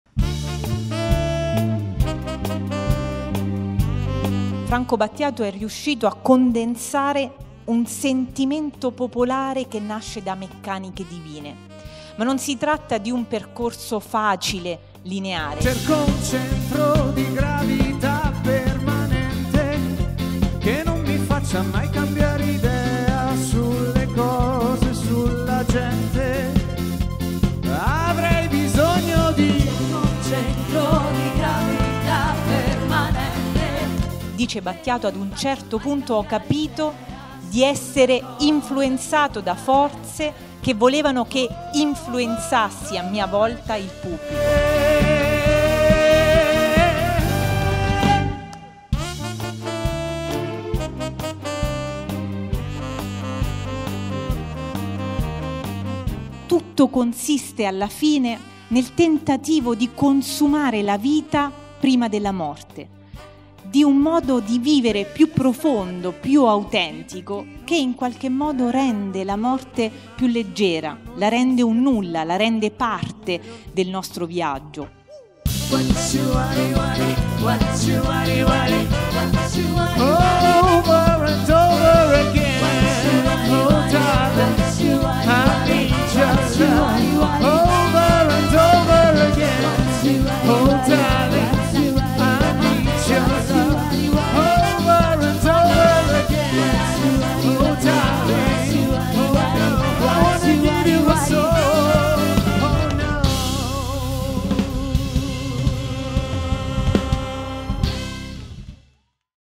Filosofia di Franco Battiato - Il Trailer
Il trailer dello spettacolo filosofico-musicale di Popsophia dedicato alla filosofia di Franco Battiato.

Ideato e diretto da Lucrezia Ercoli
con l'ensemble musicale Factory
regia video Riccardo Minnucci.